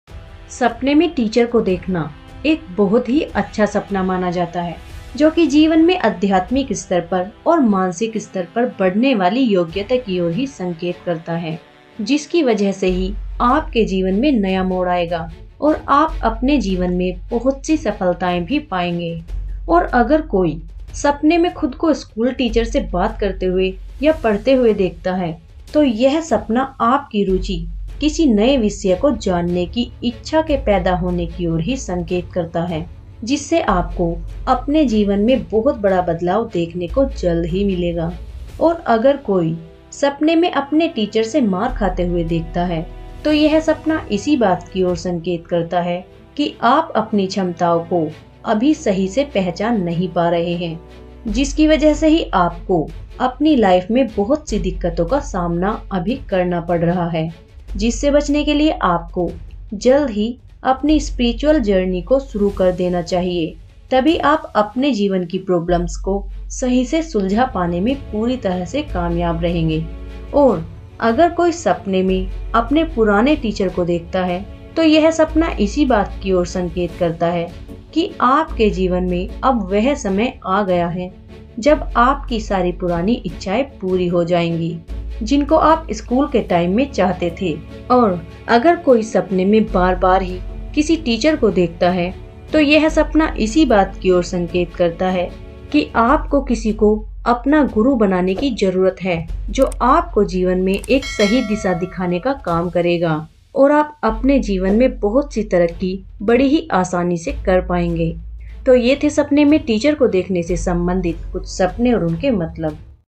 Sapne me teacher ko dekhna | seeing teacher in dream | sapne me adhyapak ko dekhna
Sapne me apne purane teacher ko dekhna ya kisi bhi adhyapak ko dekhna kitan shubh ya shubh hota hai.